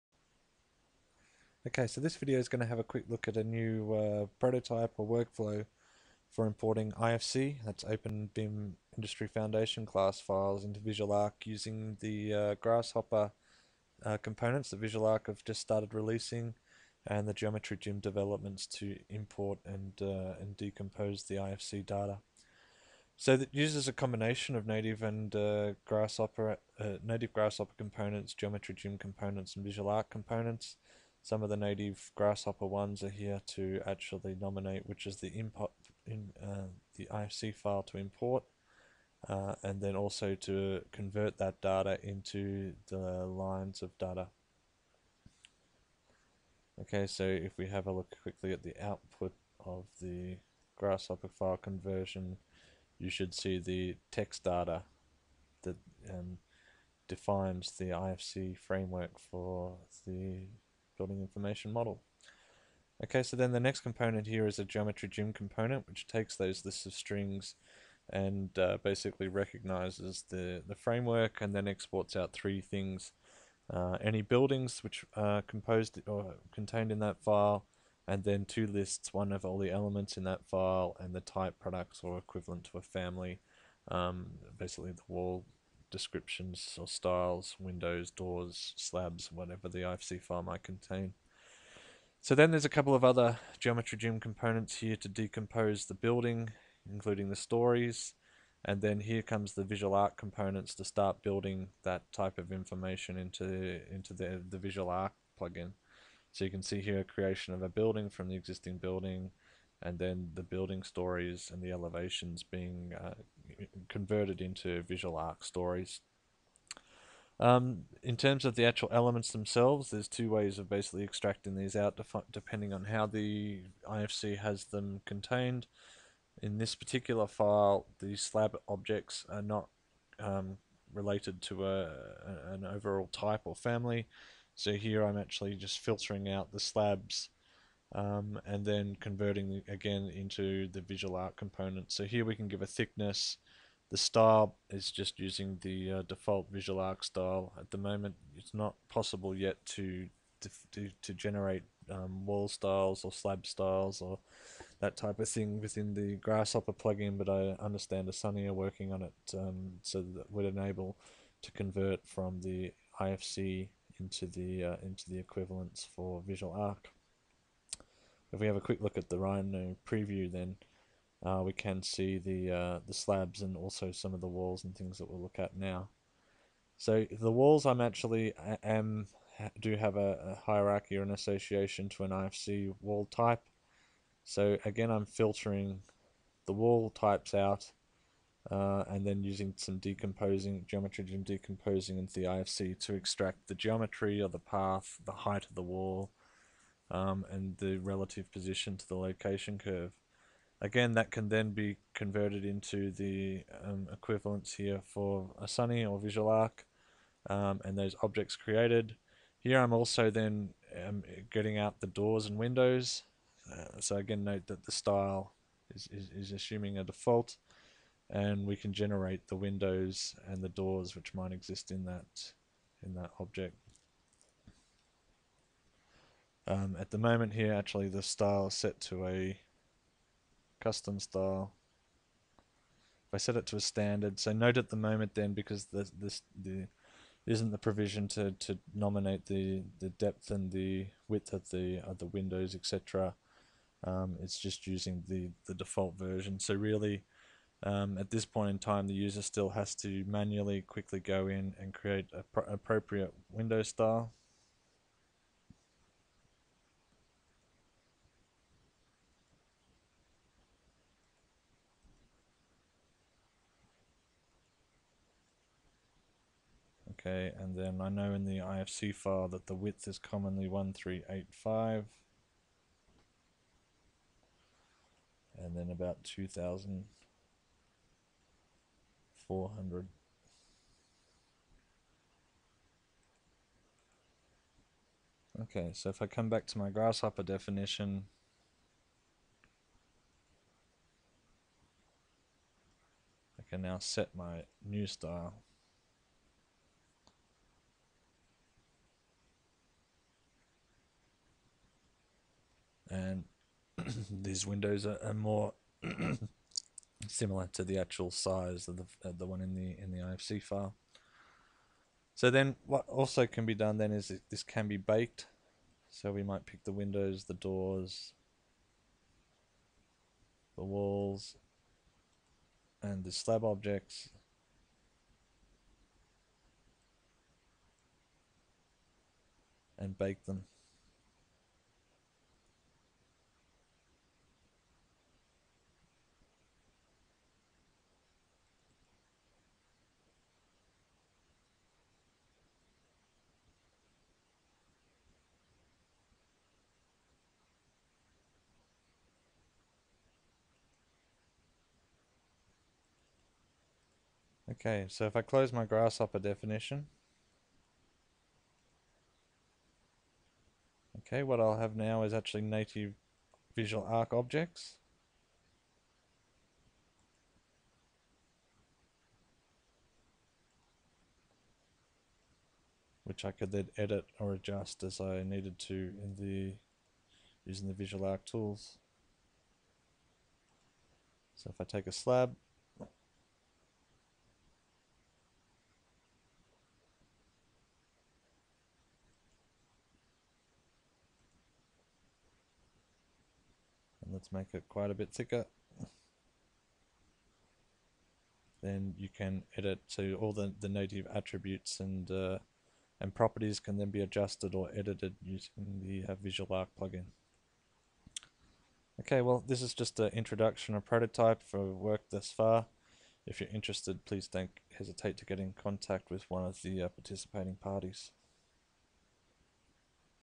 130708 IFC importing to VisualARQ via Grasshopper and Geometry Gym
A prototype to enable IFC importing to Rhino plugin VisualARQ using Grasshopper and Geometry Gym Components to transfer levels, walls, slabs, windows and doors into VisualARQ. File available